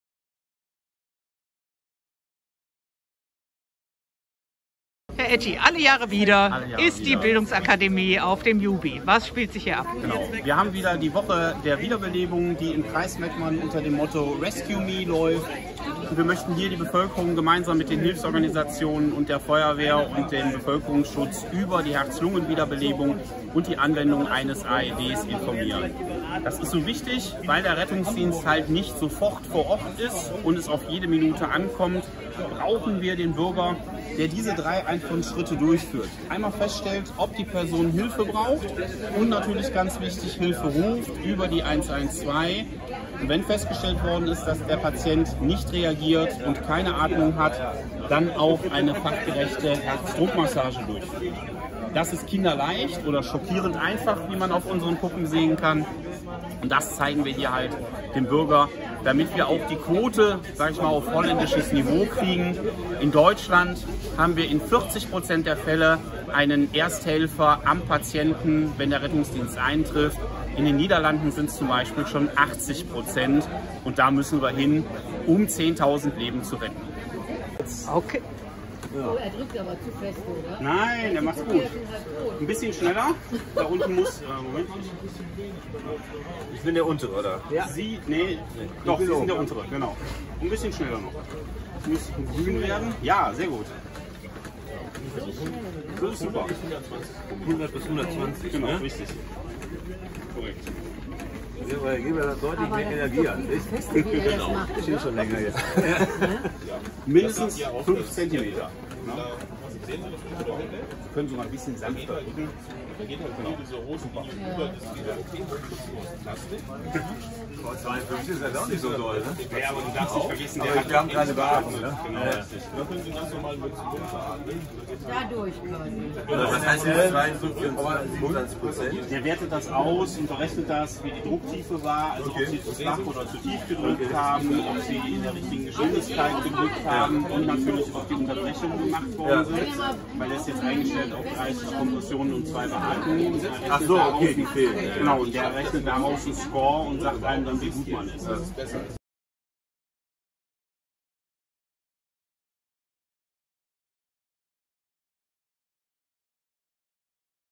Woche der Wiederbelebung (22.09.2022)
In Vertretung für Landrat Thomas Hendele hat in diesem Jahr Kreisdirektor Philipp Gilbert die bundesweite Aktion "Woche der Wiederbelebung" unterstützt und sich gestern auf dem Jubiläumsplatz selbst über lebensrettende Maßnahmen informiert. Dort hat er sich zum Beispiel zeigen lassen, wie man eine Herzdruckmassage durchführt. ❤️

Jedes Jahr erleiden mindestens 50.000 Menschen in Deutschland einen Herz-Kreislauf-Stillstand außerhalb eines Krankenhauses. Ihre Überlebenschance sinkt innerhalb weniger Minuten. ⌛ Denn die Gehirnzellen beginnen bei einem Herz-Kreislauf-Stillstand bereits nach nur drei bis fünf Minuten ohne Blutfluss unwiederbringlich zu sterben.

In diesem Zeitfenster kann jeder mit einfachen Maßnahmen dazu beitragen, Leben zu retten, indem mit einer sofortigen Herzdruckmassage begonnen und so die wichtige Zeit bis zum Eintreffen des Rettungsdienstes überbrückt wird. 🚑

Mit der zehnten bundesweiten Aktion „Woche der Wiederbelebung“ vom 19.  bis 25. September soll auf die einfachen, aber so wichtigen lebensrettenden Maßnahmen aufmerksam gemacht werden. Erneut beteiligt sich auch der Kreis Mettmann zusammen mit den Hilfsorganisationen und den Feuerwehren an dieser Aktionswoche. 👍